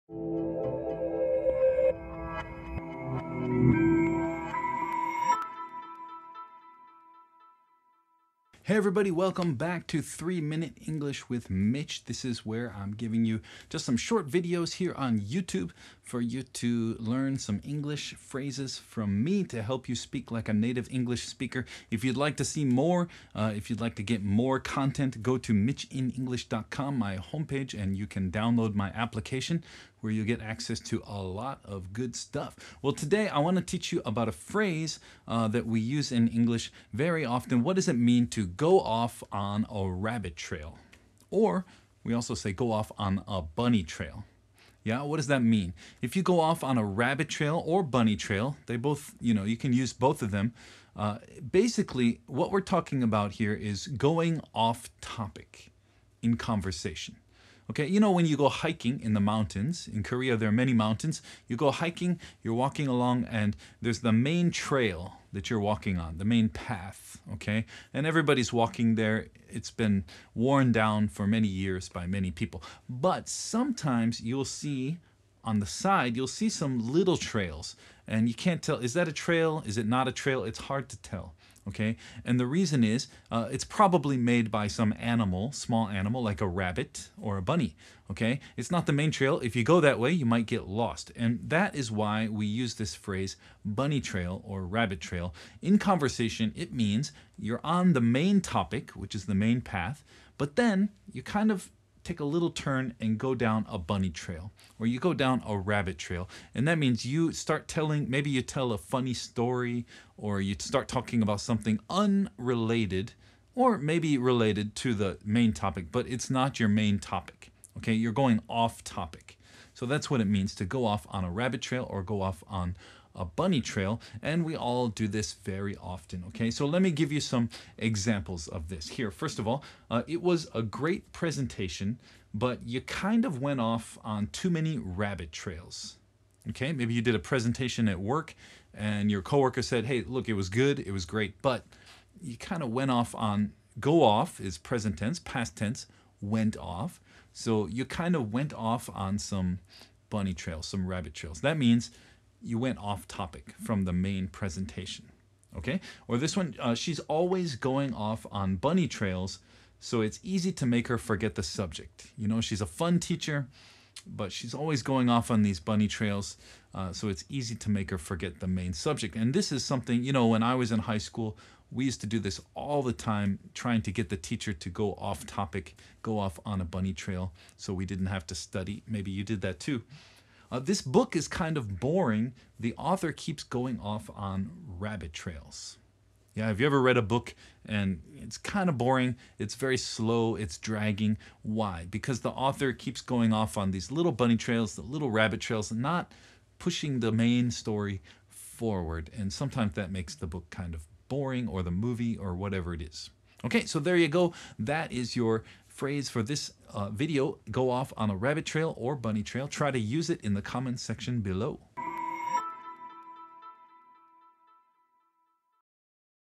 3 Minute English With Mitch - Today's Phrase: Go Off On A Rabbit / Bunny Trail
Learn how to speak English like a native English speaker!
Today's Phrase is "Go Off On A Rabbit / Bunny Trail."